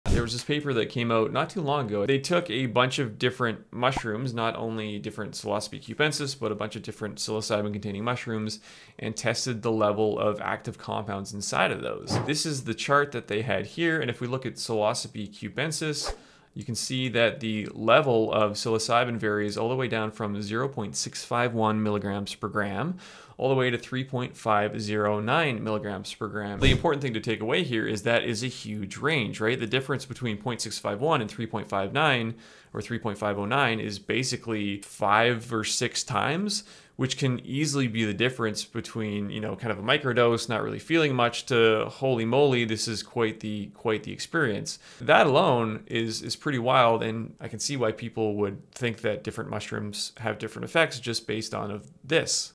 Why Taking THOSE 🍄🍄 Can Have Such Wildly Different Effects
It is pretty darn difficult to measure the potency level of psilocybin-containing mushrooms!